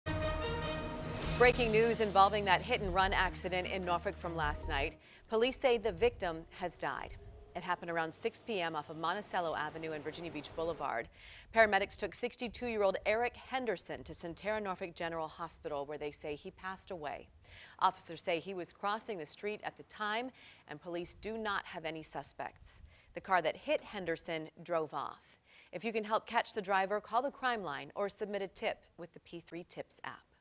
62-year-old NC man dies after hit-and-run in Norfolk
Police said the incident happened around 5:35 p.m. on Wednesday in the 1000 block of Monticello Avenue.